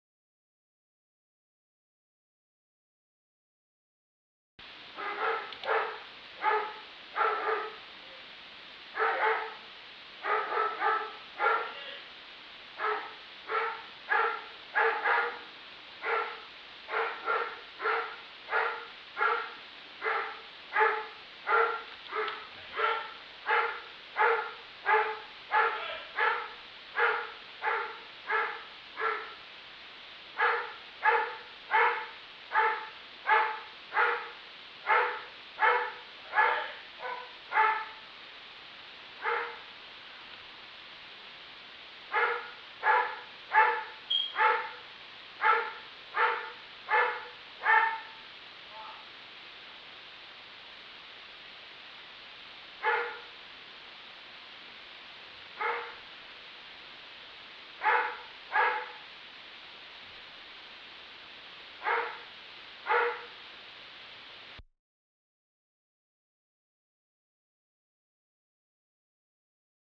My neighbor's dog barks 63 times in a single minute
this is content